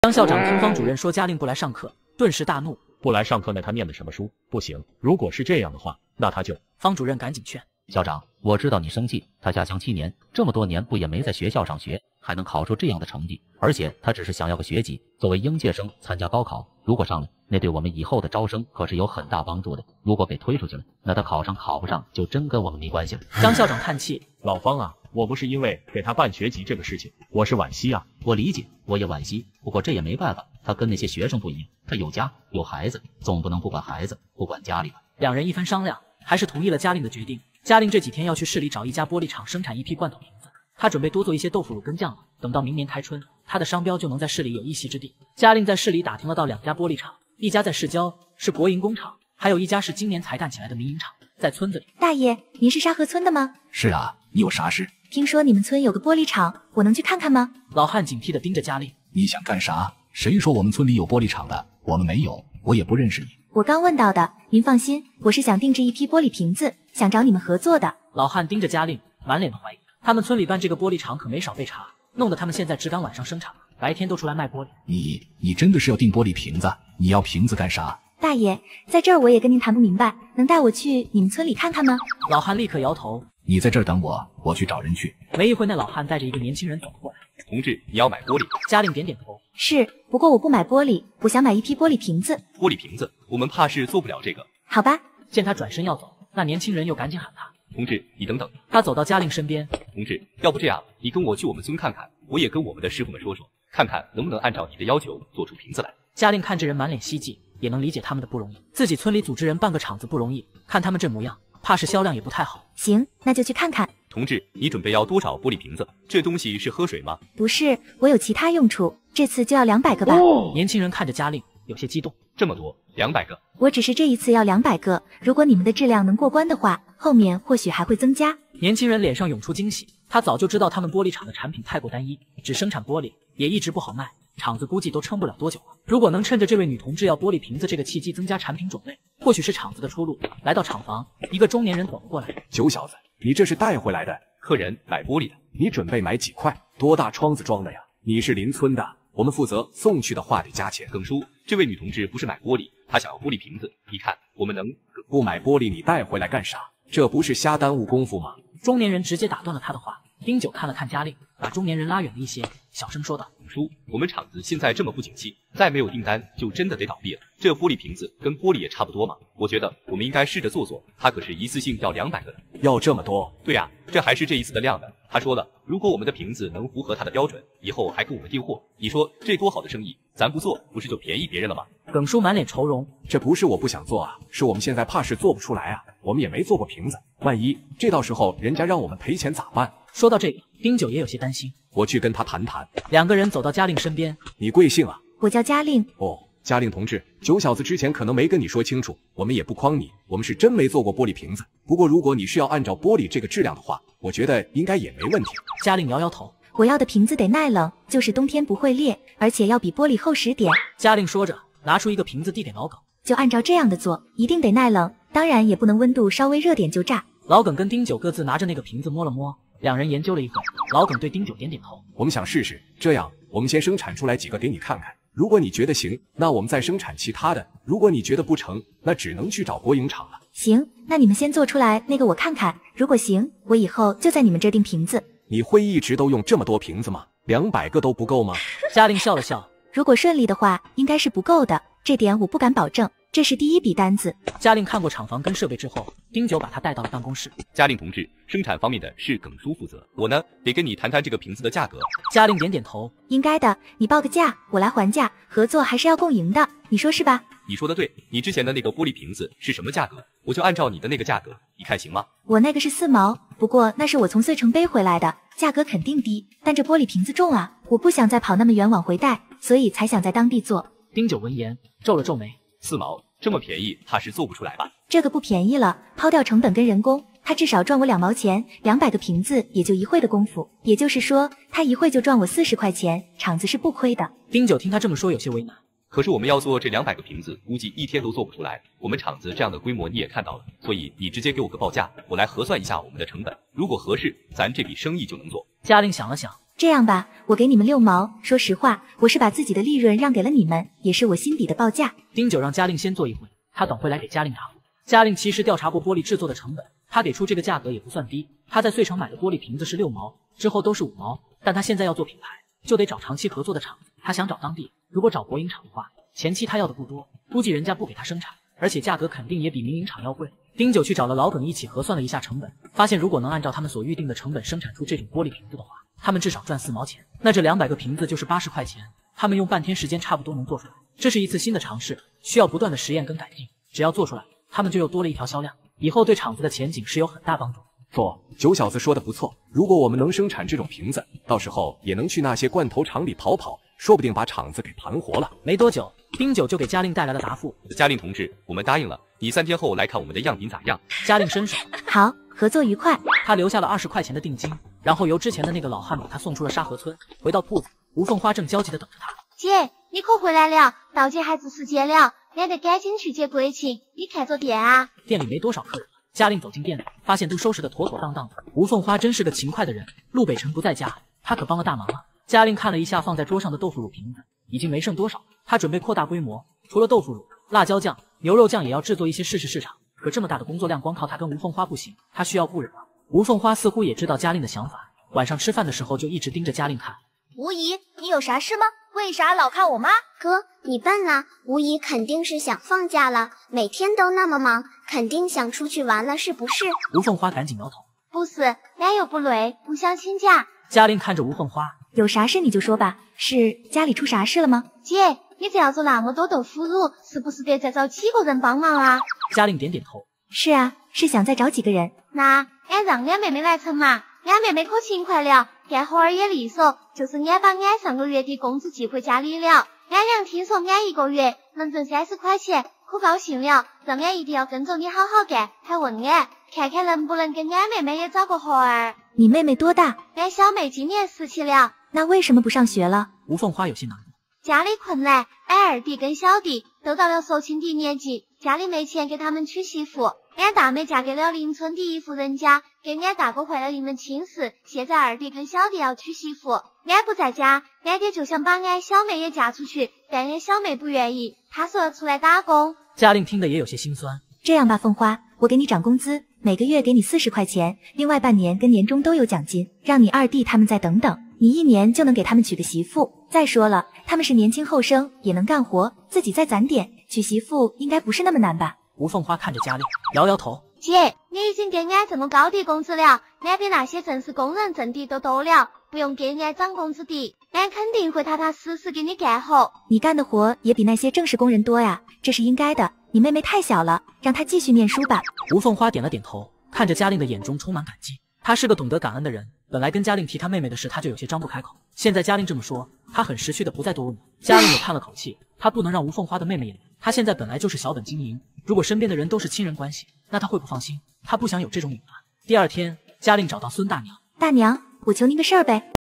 嘉玲重生记第46集：嘉炩的学籍成功办理，在外做生意时决定做出自己的品牌，扩大生产后雇佣其他人工作#重生80年代 原创动画【嘉炩动画】
哈喽，大家好~
这里是嘉炩动画
✨你快乐是我创作的源泉，让我带给你不一样的快乐😂😂